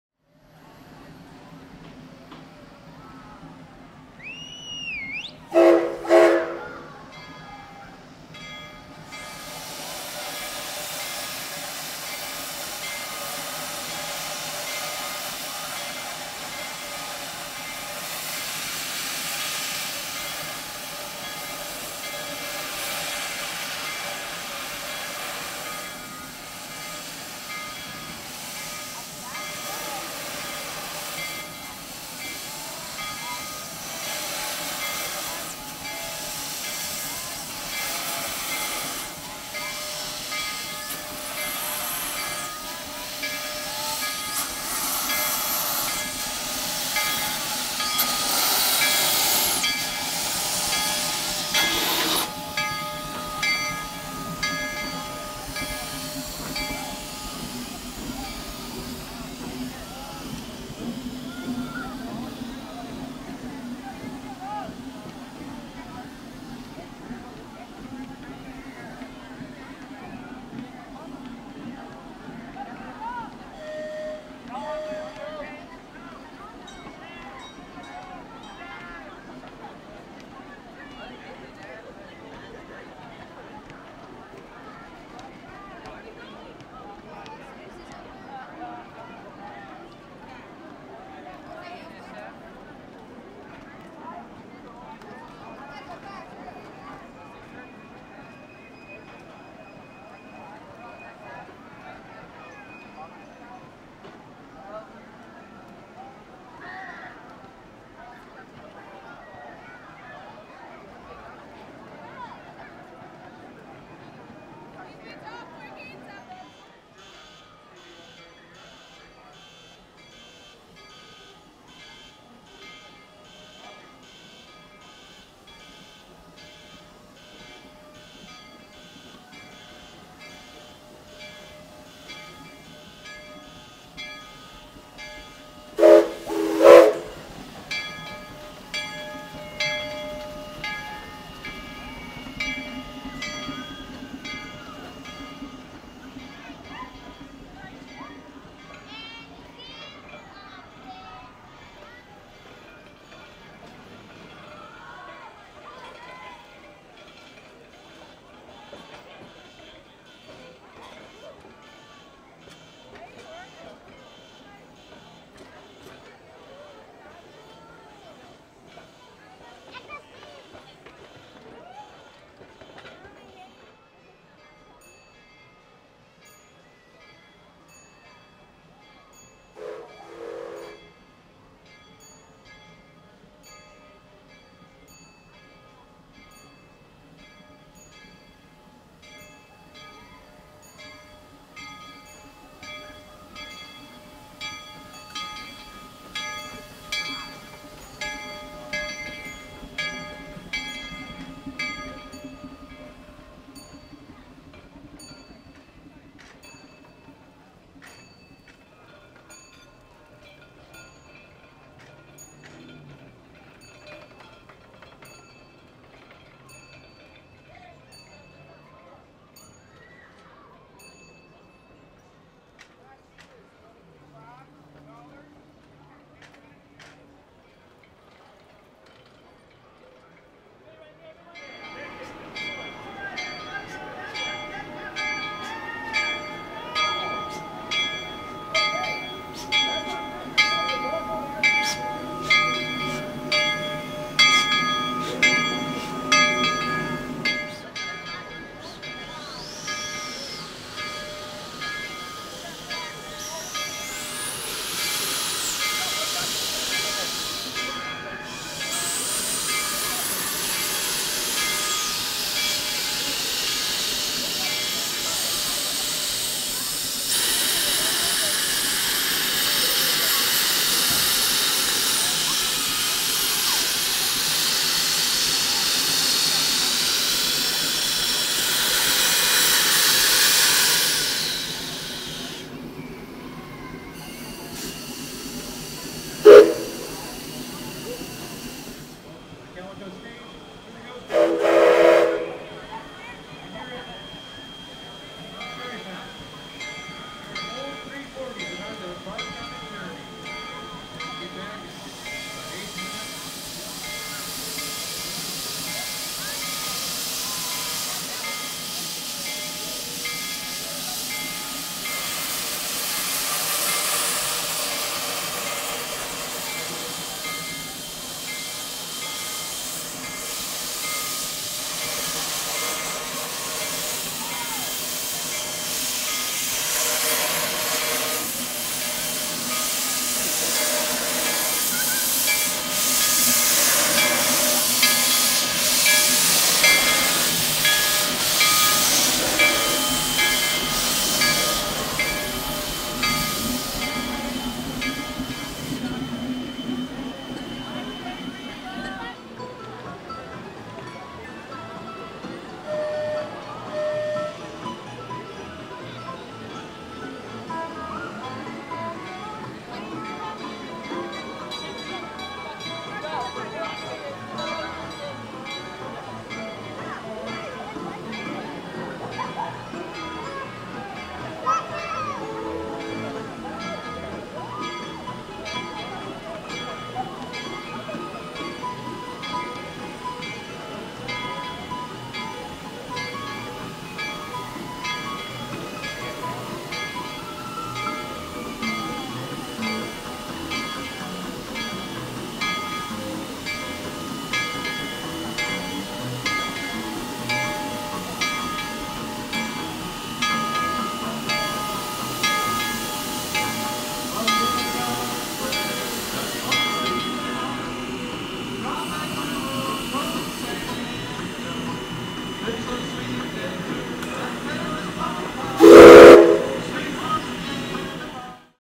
Denver & Rio Grande Western 340
D&RGW 340 operating at Knott's Berry Farm on the GT&C.